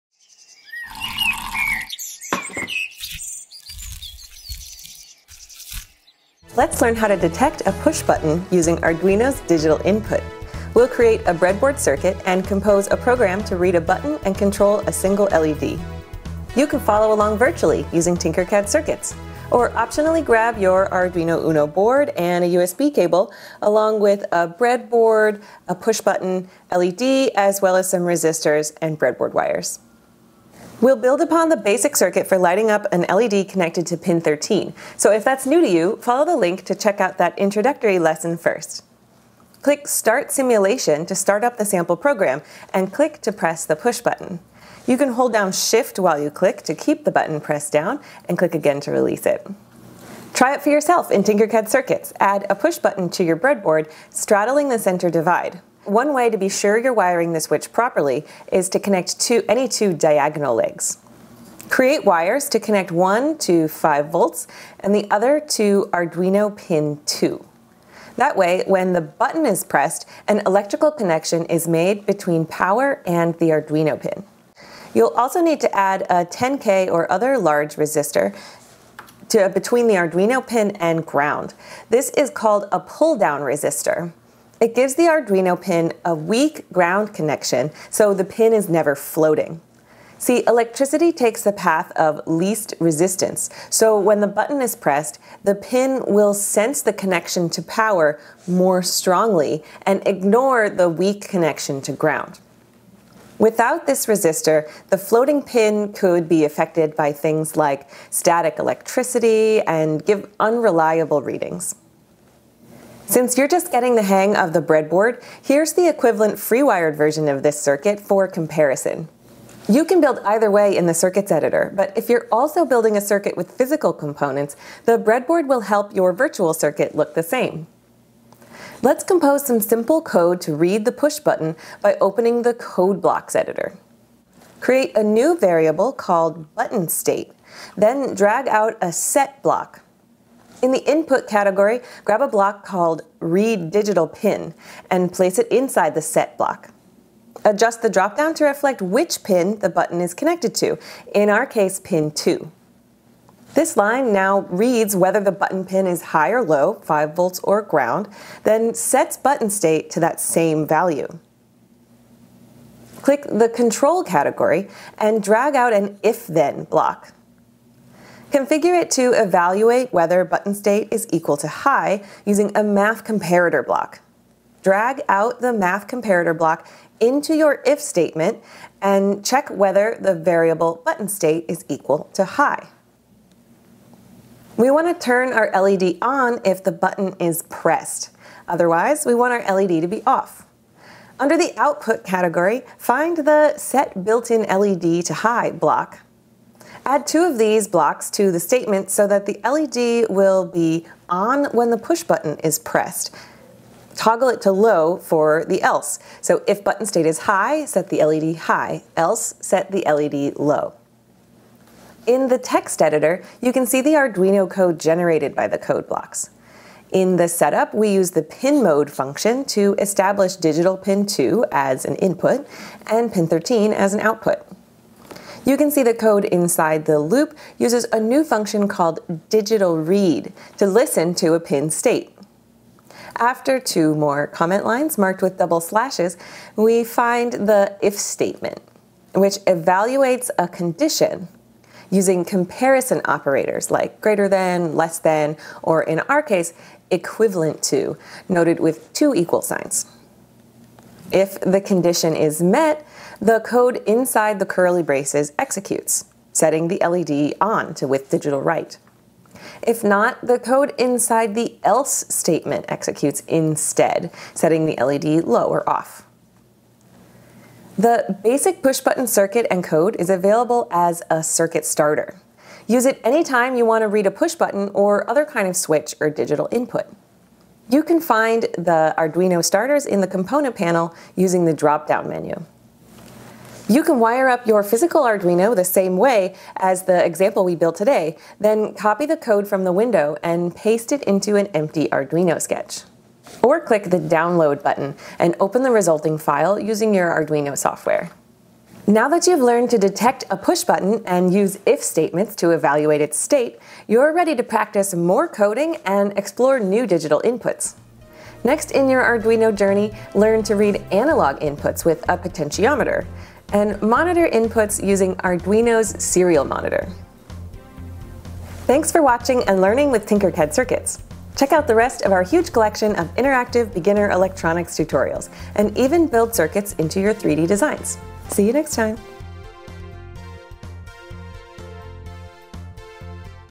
Pushbutton Digital Input With Arduino in Tinkercad
Let's learn how to read a pushbutton using Arduino's digital input! We'll connect up a simple circuit using a solderless breadboard and use some simple Arduino code to control a single LED.

Tinkercad is a free, online 3D design and 3D printing app for everyone.